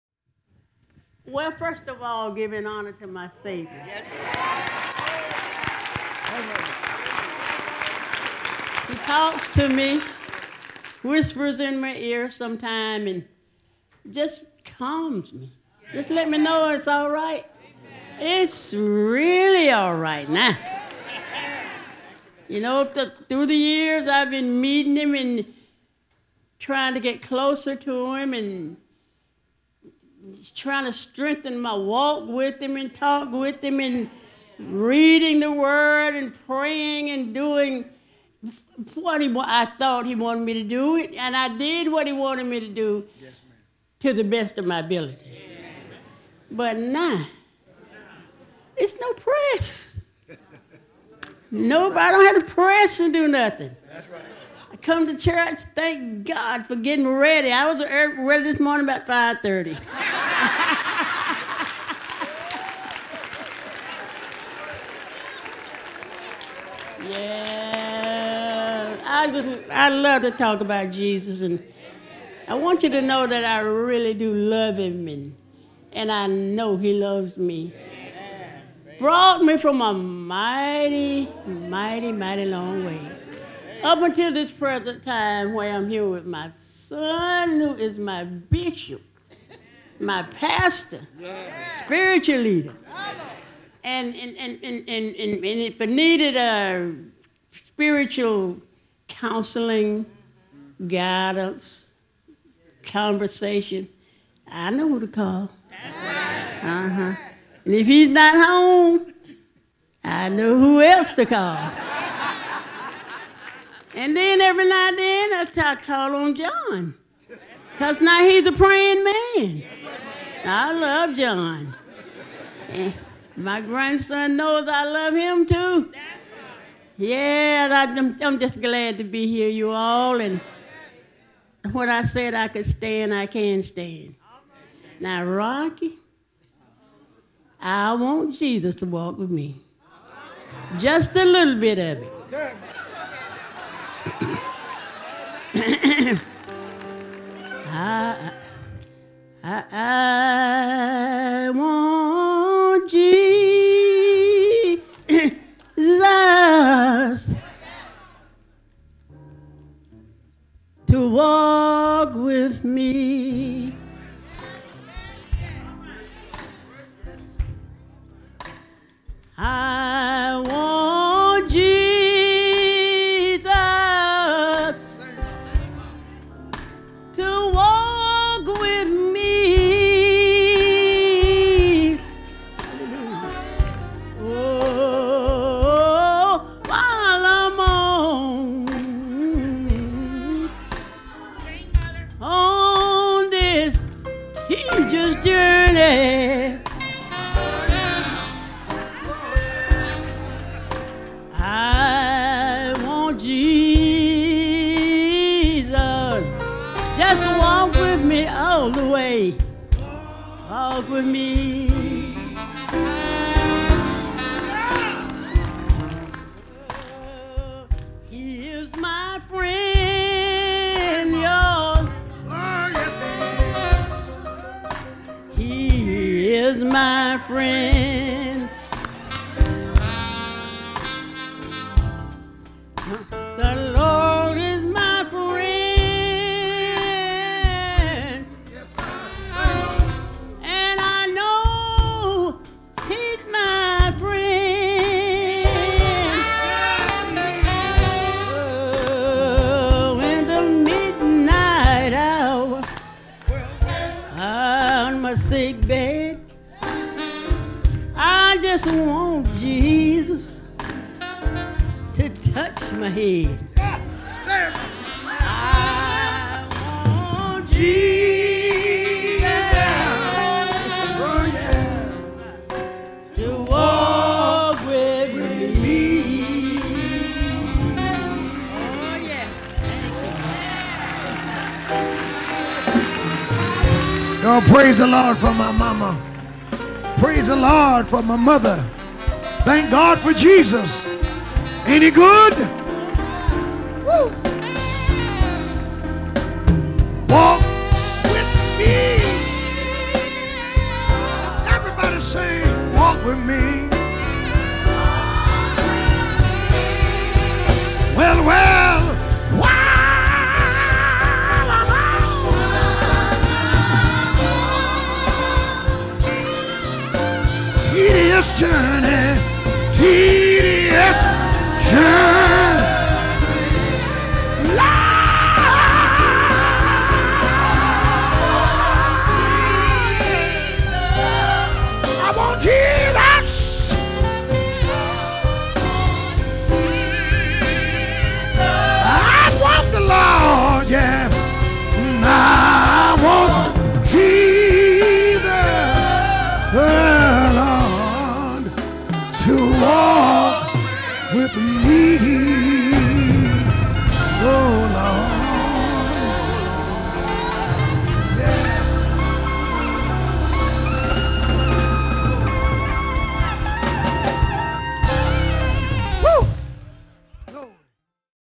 Bishop Wooden's Mother Returns to Church August 11, 2019
After an extended illness, Bishop Wooden's Mother, Gwendolyn Ellison, returned to Sunday morning worship armed with a testimony about the goodness of the Lord! Hallelujah!!